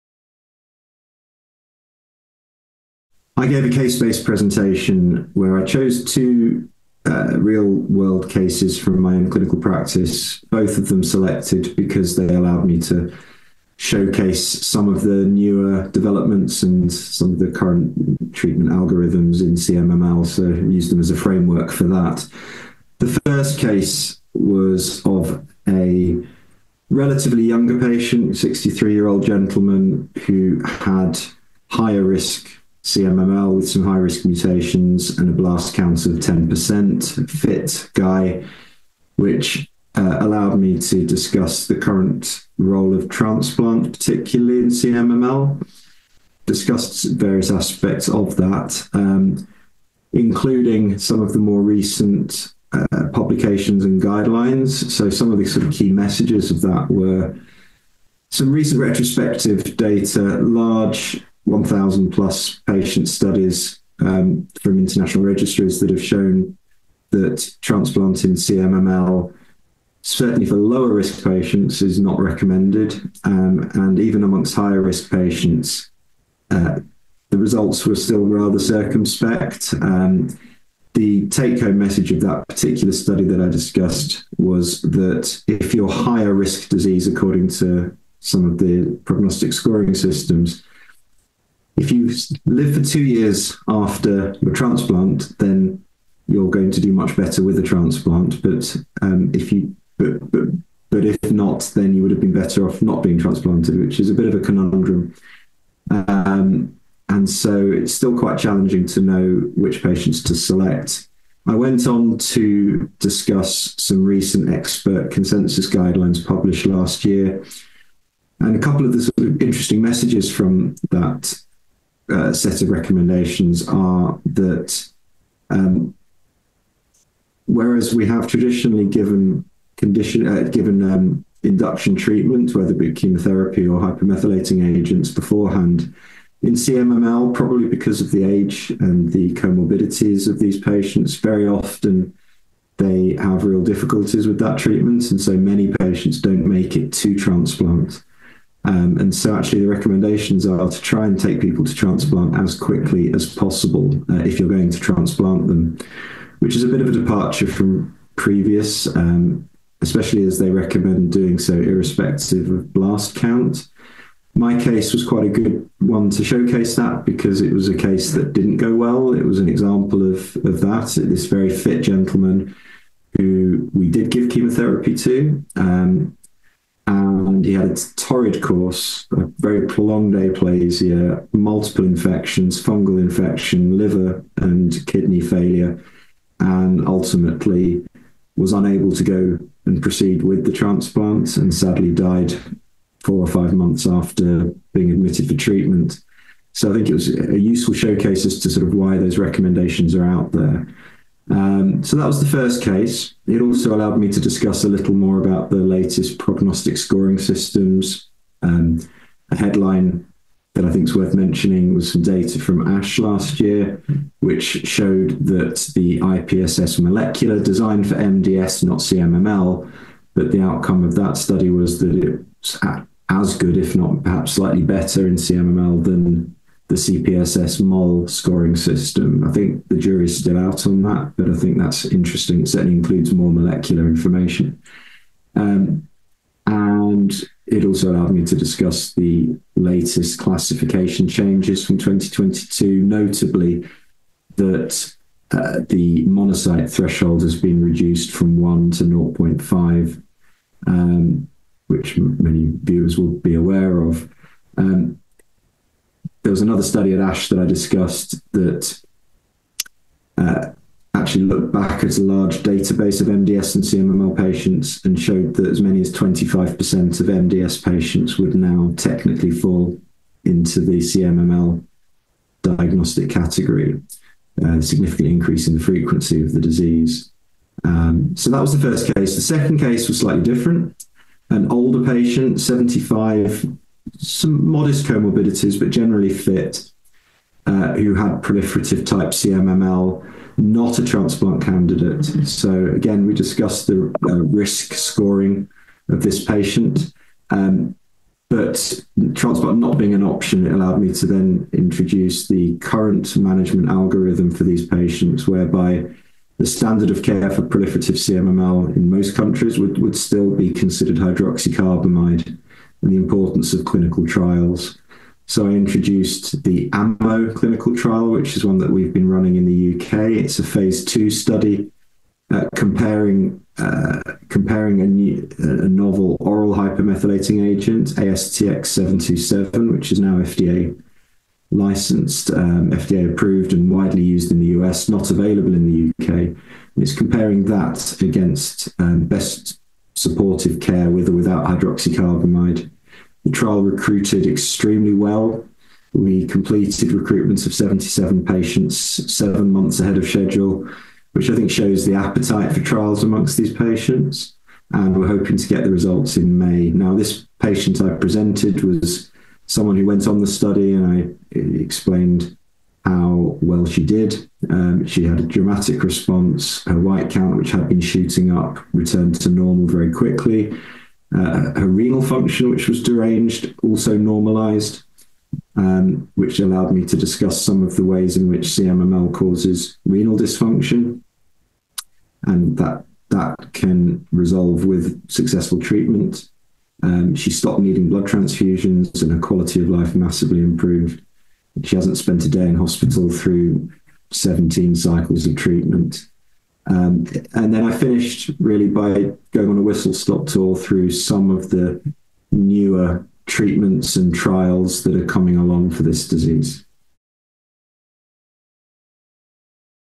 Updates in the management of CMML: a case-based presentation
In this interview, Daniel Wiseman, MBBS, PhD, University of Manchester, Manchester, UK, provides insight into a case-based presentation in which he utilized two real-world cases from his clinical practice to discuss the current management of patients with chronic myelomonocytic leukemia (CMML). Dr Wiseman highlights the challenges of determining which patients will benefit from transplant, discusses the latest prognostic scoring systems and classification changes, and mentions the positive progress made with the AMMO clinical trial (ISRCTN30808508). This interview took place at the European School of Haematology (ESH) 4th How to Diagnose and Treat: CML/MPN meeting in Vienna, Austria.